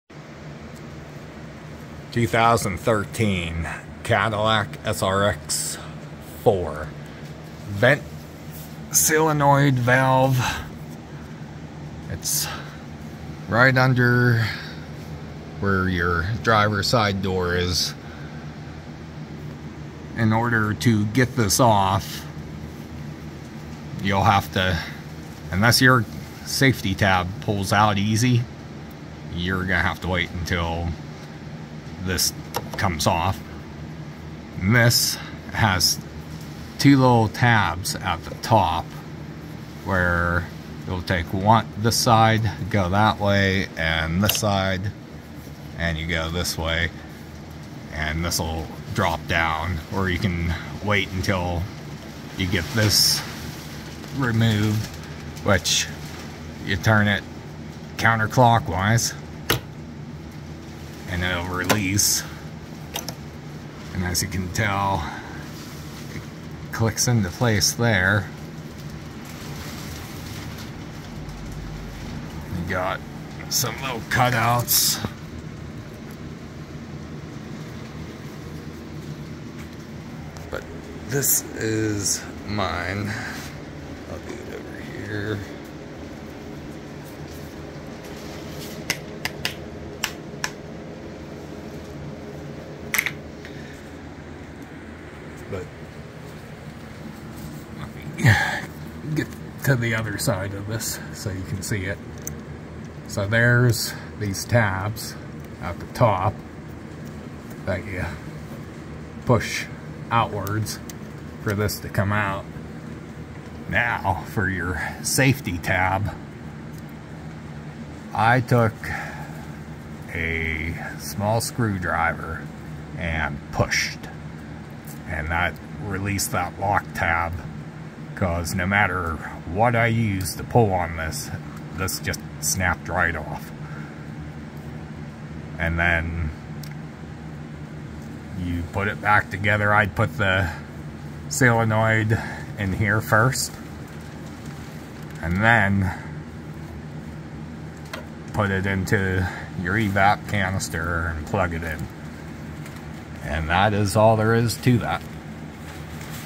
2013 Cadillac SRX4 vent solenoid replacement P0449, P0455.
This was the fix for me with these codes as well as not being able to fill the fuel tank up fast without it stopping unless you go super slow.